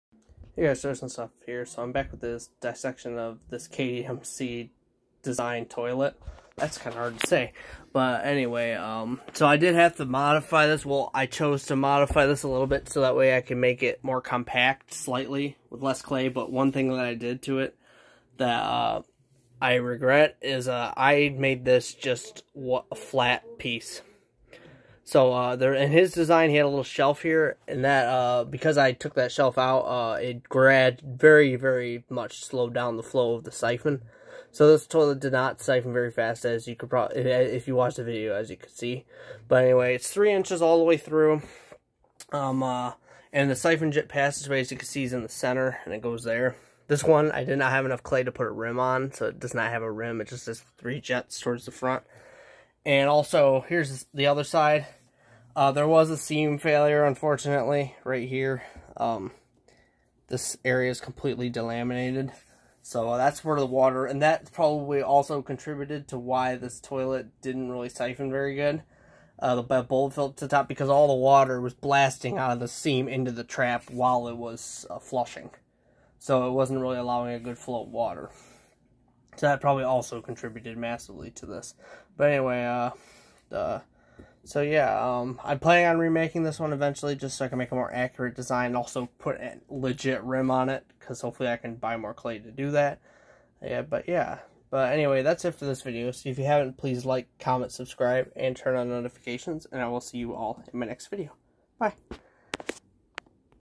KDMC Designed Toilet dessiction.
KDMC designed this particular model. I plan on re-making it as I made some modifications that may have hindered it's performance, and also I ran out of clay and was unable to add a rim. Other than that though, it worked. Also check out KDMCs channel he's awesome.